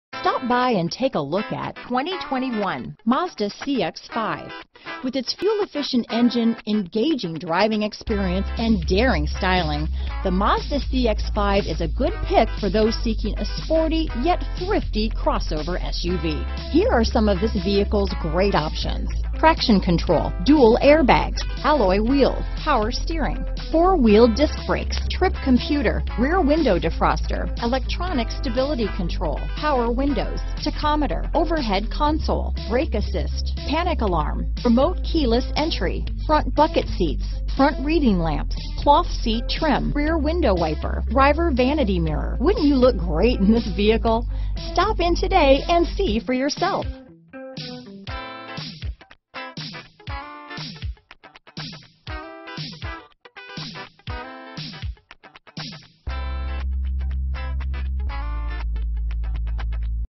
2021 Mazda CX-5 Morrow, Peachtree City, Newnan, McDonough, Union City, GA M28238
Snowflake White Pearl Mica New 2021 Mazda CX-5 available in Morrow, Georgia at Hennessy Mazda Buick GMC. Servicing the Peachtree City, Newnan, McDonough, Union City, GA area.